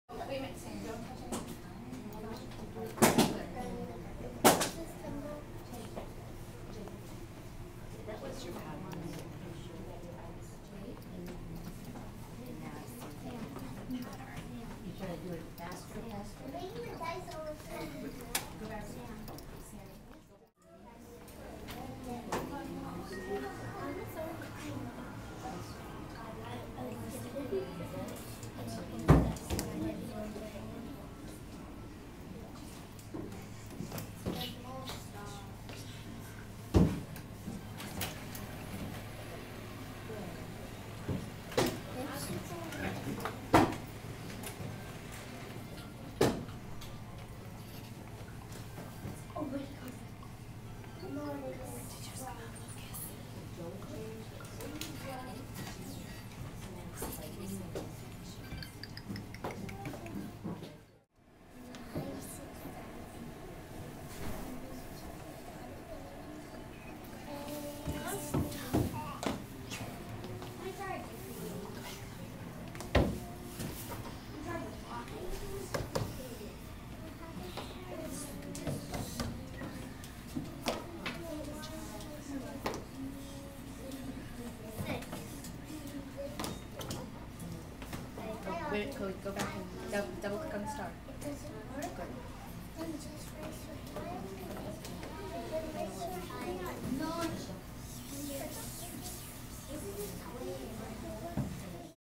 Kindergarten Tech Integration
Mrs. Kimball's class heads to the computer lab to use technology for their math centers for the day. In this video you will see students playing the "Dice Race Game" on the SMART board, "Dots 4 Tots Free" for shape recognition and "Ticki-Park" for memory skills on the iPads, and they will use Kidpix to complete a pattern assessment.